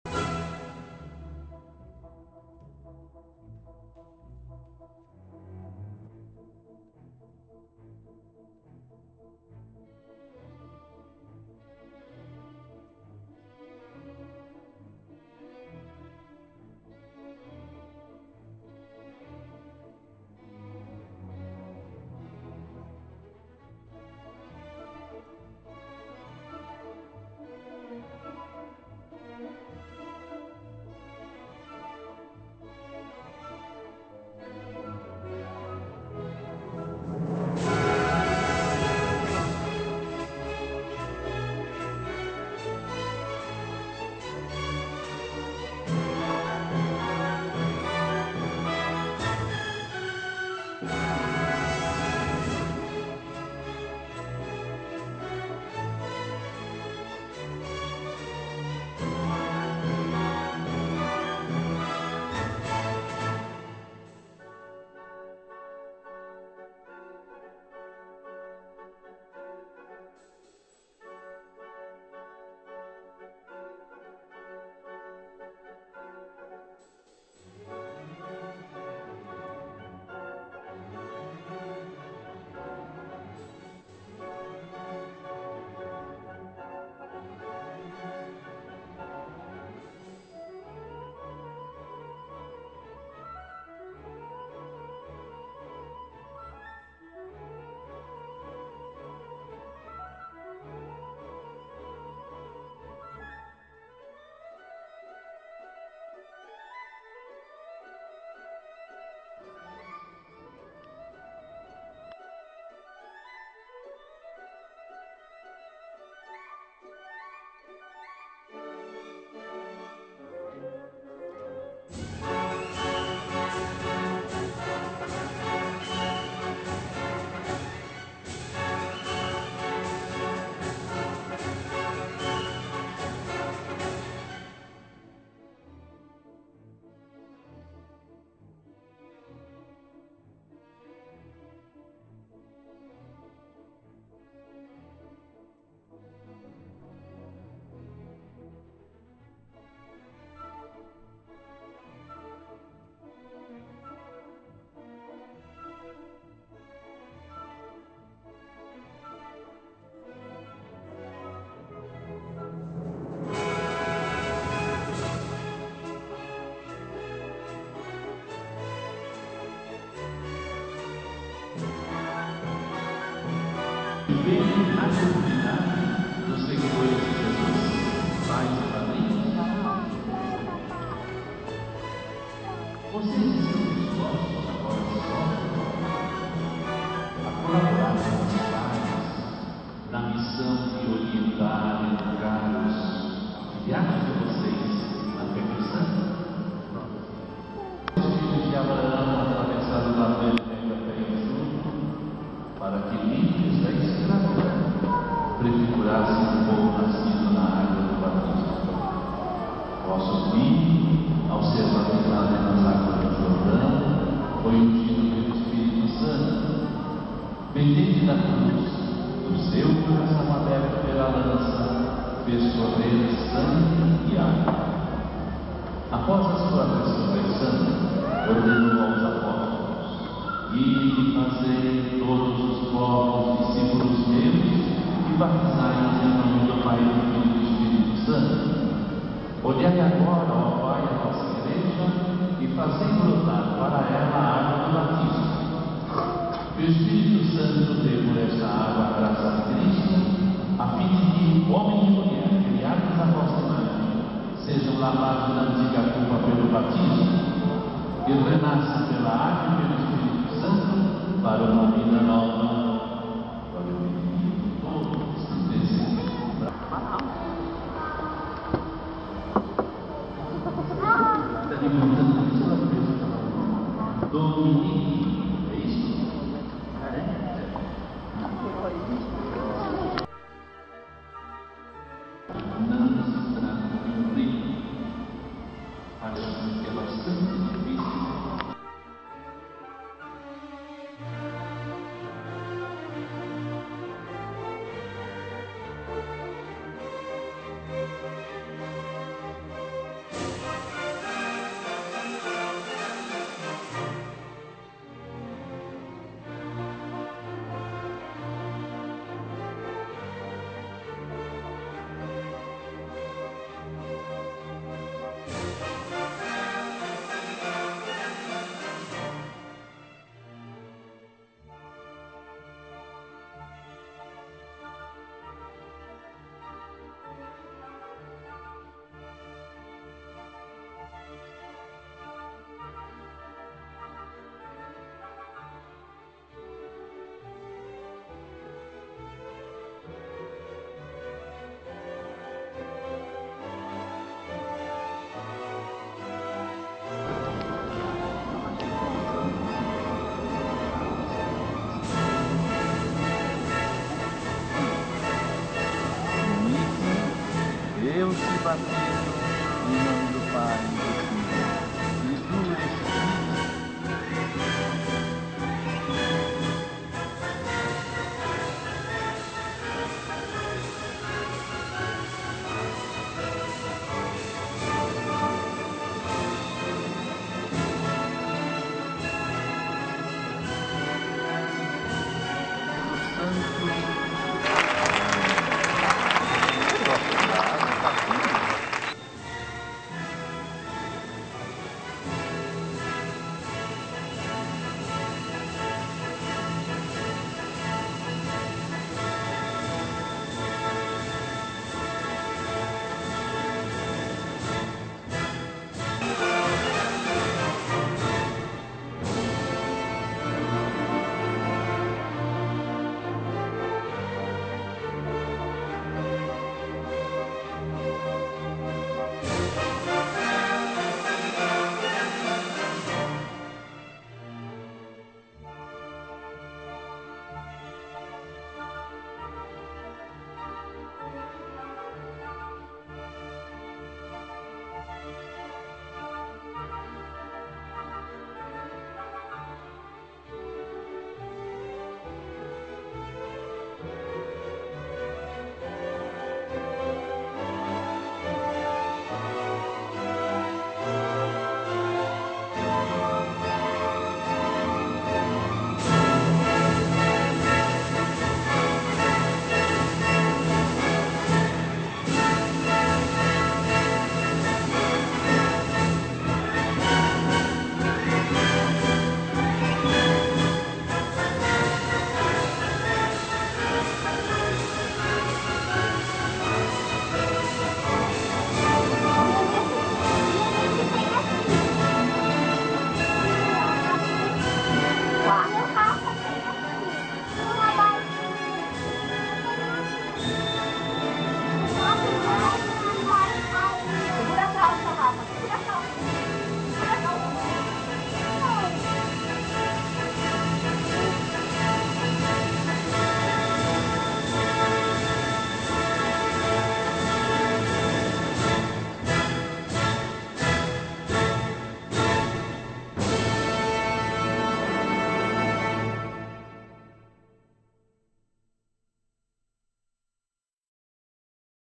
O Batizado
DÊ - Ó - EME - I - ENE - I - CÊ,  isto é, DOMINIC. VOCÊ JÁ TINHA VISTO O BATIZADO (aquele que recebe o sacramento) SOLETRAR O NOME?

Paróquia da Imaculada Conceição
São Paulo -- Brasil
23 de outubro de 2011

O Batismo é entendido como o sacramento

que abre as portas da vida cristã ao batizado,

incorporando-o à comunidade católica,

ao grande Corpo Místico de Cristo,

que é a Igreja em si.

Este ritual de iniciação cristã é feito normalmente

com água sobre o batizando, através de imersão,

efusão ou aspersão.

Ou, utilizando outras palavras do Compêndio

do Catecismo da Igreja Católica,

"o rito essencial deste sacramento consiste

em imergir na água

o candidato ou em derramar a água sobre a sua cabeça,

enquanto é invocado o Nome do Pai e do Filho

e do Espírito Santo".

O Batismo significa imergir

"na morte de Cristo e ressurgir com Ele como nova criatura".

Produção:
MARIO SIDNEI MOREIRA
24 de outubro de 2011